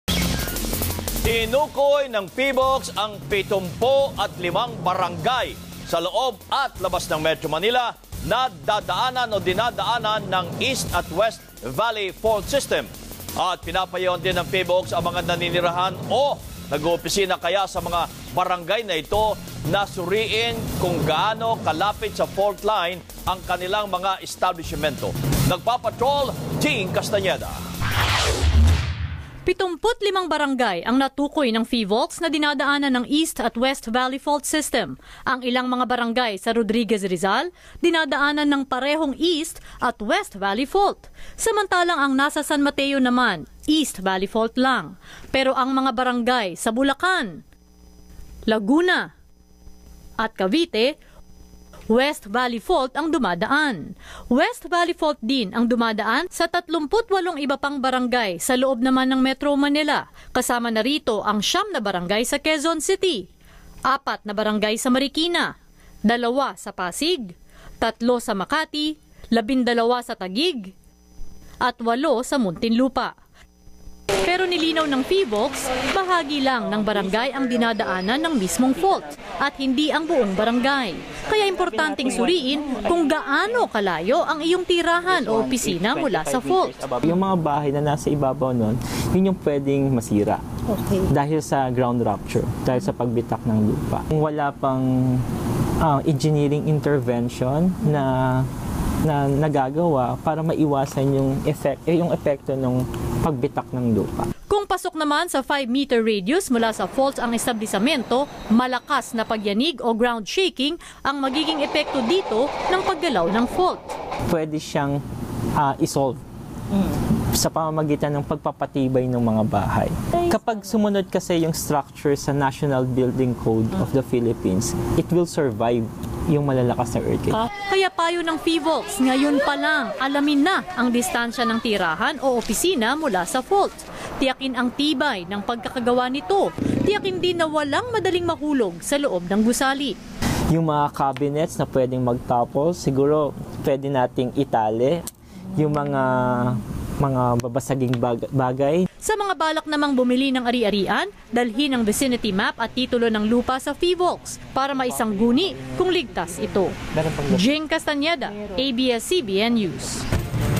Mga barangay na malapit sa East, West Valley fault systems
Tinukoy ng Philippine Institute of Volcanology and Seismology (PHIVOLCS) ang pitompu't-limang (75) barangay sa loob at labas ng Metro Manila na dinadaanan ng East at West Valley fault systems. Pinapayuhan din ng PHIVOLCS ang mga naninirahan o nago-opisina sa mga barangay na ito na suriin kung gaano kalapit sa fault line ang kanilang mga establisyimento.